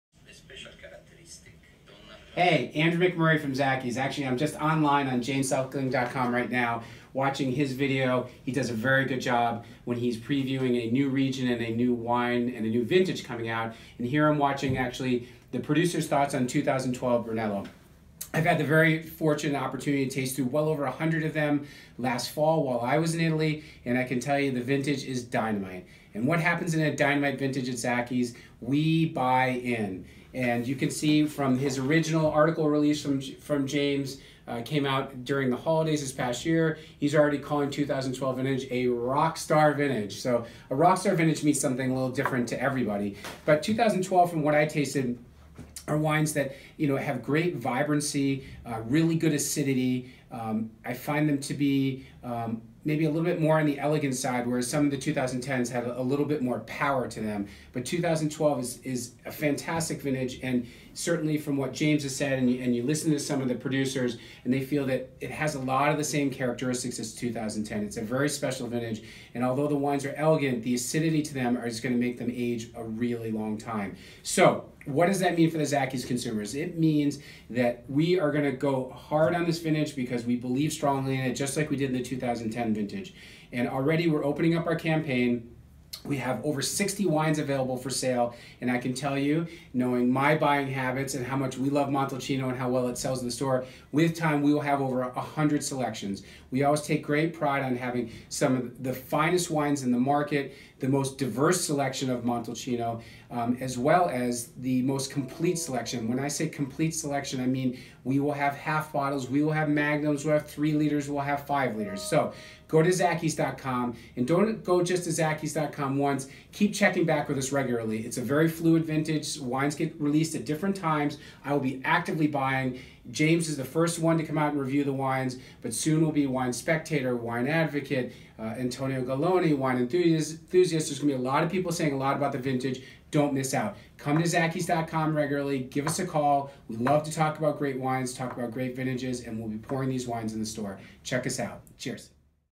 Andrew Comments on 2012 Brunello di Montalcino: A "Rockstar" Vintage
Is 2012 Brunello di Montalcino really a "rockstar" vintage? Watch to learn Andrew's thoughts.